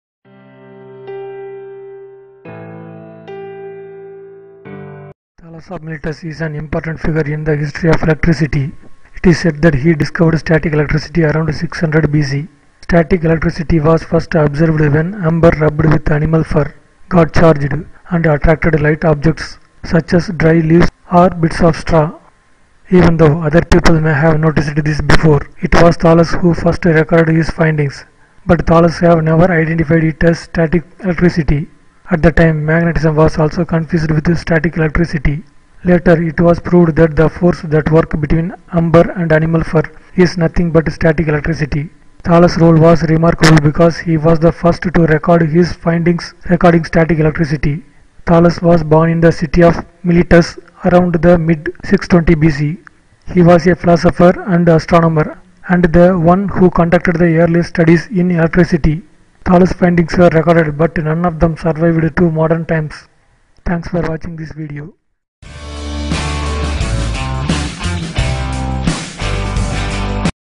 Thales of Miletus played a major role in the history of electricity
Thales of Miletus is an important figure in the history of electricity. It is said that he discovered static electricity around 600 BC. Static electricity was first observed when amber, rubbed with animal fur, got charged and attracted light objects such as dry leaves, or bits of straw.

Even though other people may have noticed this before, it was Thales who first recorded his findings. But Thales has never identified it as static electricity. At that time magnetism was also confused with static electricity. Later, it was proved that the force that work between amber and animal fur is nothing but static electricity.

Thales role was remarkable because he was the first to record his findings regarding static electricity. Thales was born in the city of Miletus around the mid 620 BC. He was a philosopher, and astronomer and the one who conducted the earliest studies in electricity.

Thales findings were recorded but none of them survived to modern times.